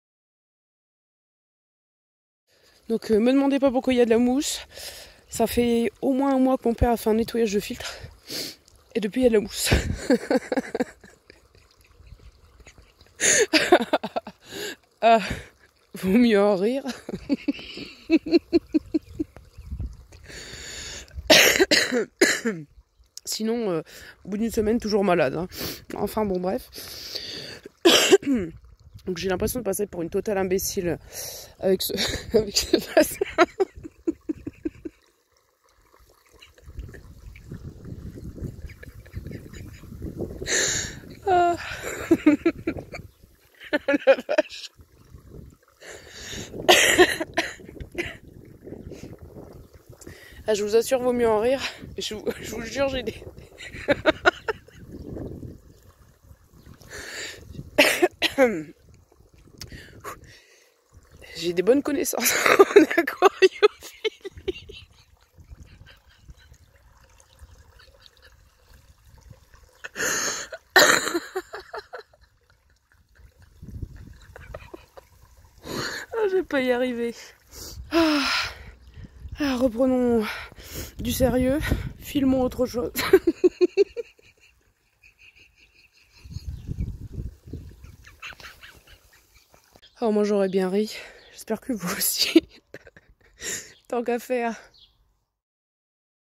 Fou rire au bassin 🤣
Rien que pour le plaisir, un extrait de la vidéo N°51, seulement le bon fou rire... !!